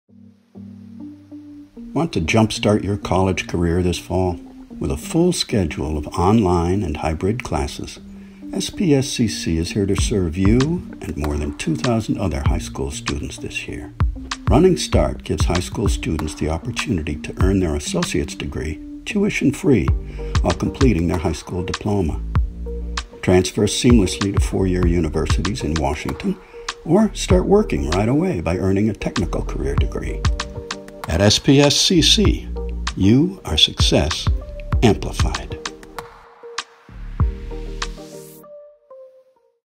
Enroll in Running Start
High school junior or senior? Running Start is the way to go this year. Enroll in tuition-free college classes and earn college credits, taught online by experienced instructors. spscc.edu/runningstart